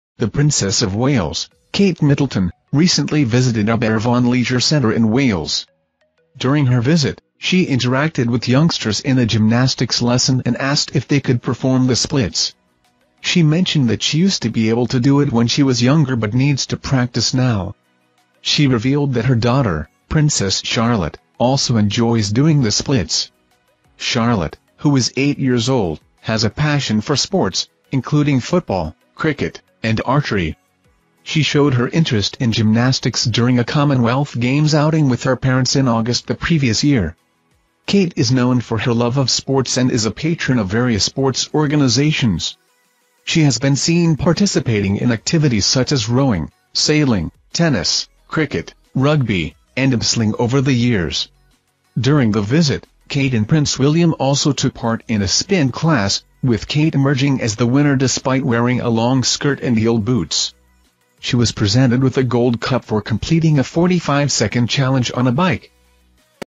Secret skill Princess Charlotte shares with mum Kate Middleton that's very hard to do
The Princess of Wales is known for her athletic hobbies and disclosed a hidden skill which she shares with daughter Princess Charlotte during a recent trip to a sports centre in South Wales